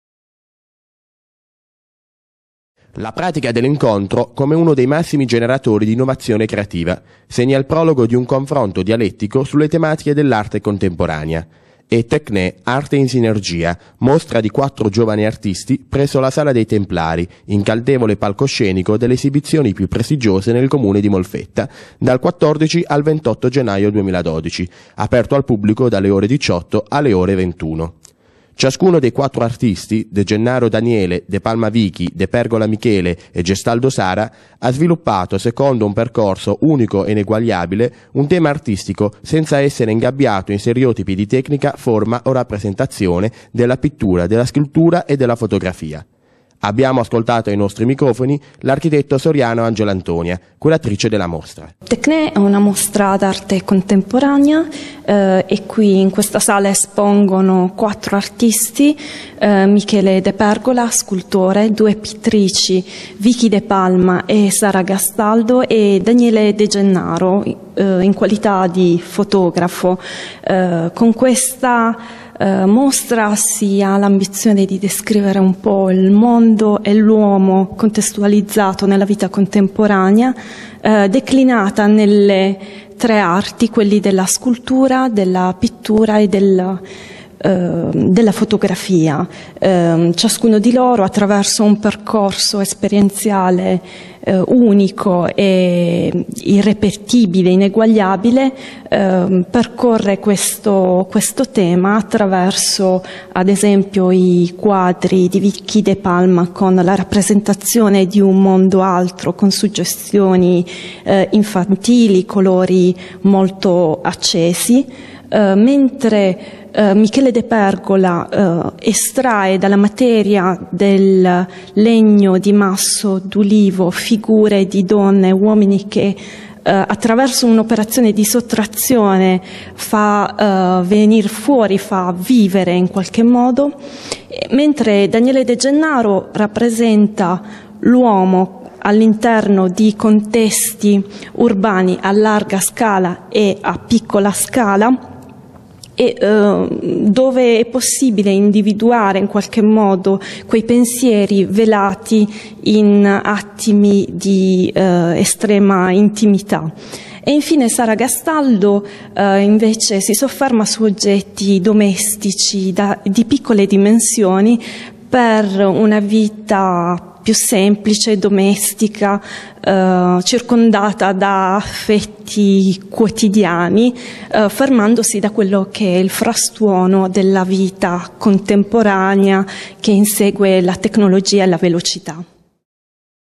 MOLFETTA. Arte in sinergia
Molfetta - La pratica dell'incontro, come "uno dei massimi generatori di innovazione creativa", segna il prologo di un confronto dialettico sulle tematiche dell'arte contemporanea. È "Techné, arte in sinergia", mostra di quattro giovani artisti, presso la Sala dei Templari, incantevole palcoscenico delle esibizioni più prestigiose nel comune di Molfetta, dal 14 al 28 gennaio 2012, aperto al pubblico dalle ore 18.00 alle 21.00.

Ciascun dei quattro artisti, de Gennaro Daniele, Depalma Vicky, Depergola Michele e Gastaldo Sara, ha sviluppato, secondo un percorso esperienziale unico e ineguagliabile, un tema artistico, senza essere ingabbiato in stereotipi di tecnica, forma o rappresentazione, della pittura, della scultura e della fotografia.

de Gennaro Daniele, fotografo: il linguaggio della tecnica espressiva della fotografia si sviluppa intorno alla struttura figurativa. Il tema centrale delle opere è l'intimità velata dei pensieri delle figure umane, espressa da sguardi disattesi, dalla quotidianità dei gesti reiterati. Raccontano i drammi, le pene, le aspettative nascoste all'occhio dell'osservatore, in cui, tuttavia, è semplice immedesimarsi.

Depalma Vicky, scenografa di formazione e fotografa d'adozione: coltiva l'arte della fotografia come l'esigenza per la scoperta di un mondo "diversamente reale". I suoi quadri sono un mix di materia e immagini che invitano l'osservatore a un  viaggio all'interno di un mondo in cui non esiste regola, tutto è reale nell'irreale, e le città hanno nomi di donna.

Depergola Michele, scultore: nella sua bottega realizza sculture in legno di ulivo, simbolo della sua terra: la Puglia. Le sculture sono rigorosamente realizzate a mano utilizzando scalpello e mazzuolo. L'artista,  assecondando l'espressività del legno,  estrapola dalla materia, uomini e donne:  anime libere da legami e da tormenti.

Gastaldo Sara, pittrice: le sue miniature, oggetti contemporanei contenuti in piccoli cartoni telati, descrivono un mondo a una dimensione medioevale della vita in cui l'esistenza individuale è circoscritta all'interno di una fortezza isolata, ma ricca di persone che interagiscono tra loro: sottile polemica ad un mondo contemporaneo dove la visibilità televisiva, le grandi opere infrastrutturali soffocano l'individualità a vantaggio di un omogeneità universale.